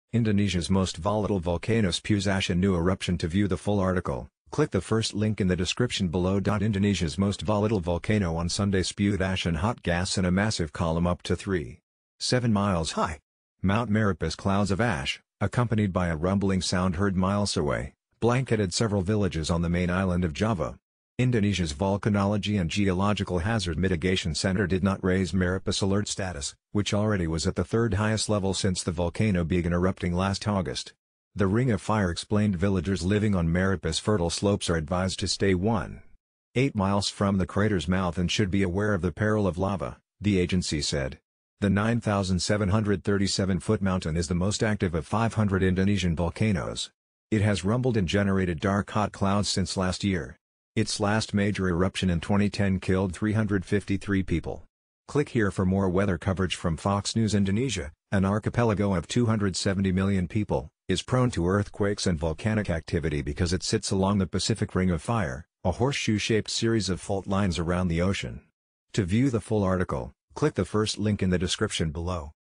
Indonesias most volatile volcano spews ash in new eruption
Indonesias most volatile volcano on Sunday spewed ash and hot gas in a massive column up to 3. 7 miles high. Mount Merapis clouds of ash, accompanied by a rumbling sound heard milesaway, blanketed several villages on the main island of Java. Indonesias Volcanology and Geological Hazard Mitigation Center did not raise Merapis alert status, which already was at the third-highest level since the volcanobegan erupting last August. THE "RING OF FIRE EXPLAINEDVillagers living on Merapis fertile slopes are advised to stay 1. 8 milesfrom the craters mouth and should be aware of the peril of lava, the agency said. The 9,737-footmountain is the most active of 500 Indonesian volcanoes.  It has rumbled and generated dark hot clouds since last year. Its last major eruption in 2010 killed 353 people. CLICK HERE FOR MORE WEATHER COVERAGE FROM FOX NEWSIndonesia, an archipelago of 270 million people, is prone to earthquakes and volcanic activity because it sits along the Pacific Ring of Fire, a horseshoe-shaped series of fault lines around the ocean.